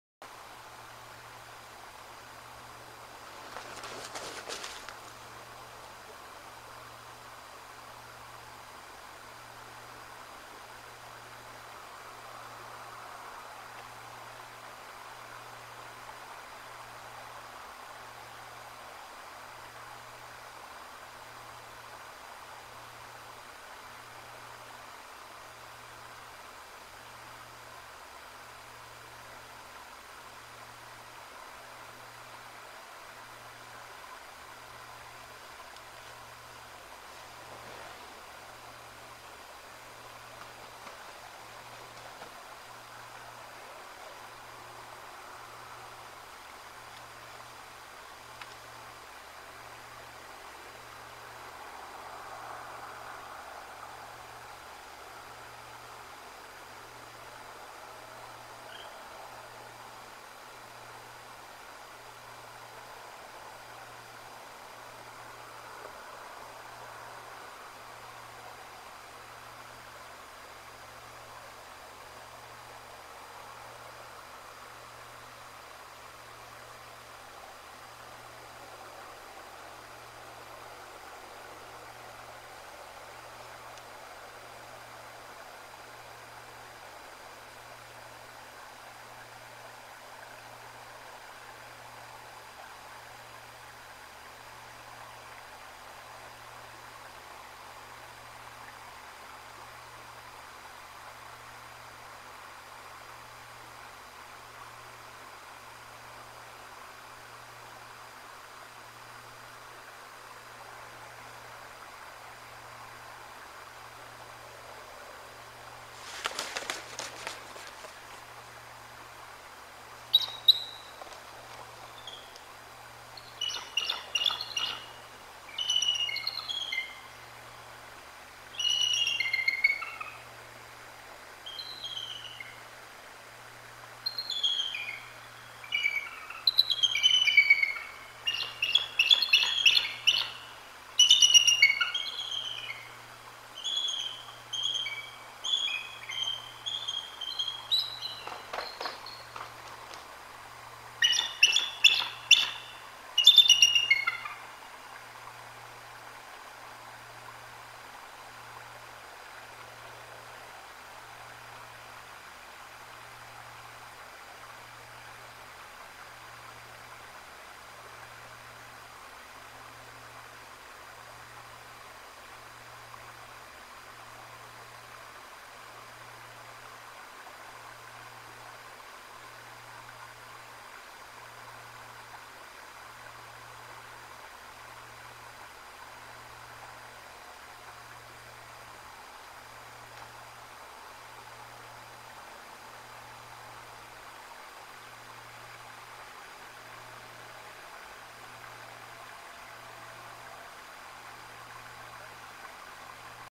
Decorah Eagles North 2/1/24 Last check after a hard day of work.
@ExploreLiveNatureCams Decorah Eagles North Nest

live stream @RaptorResourceProject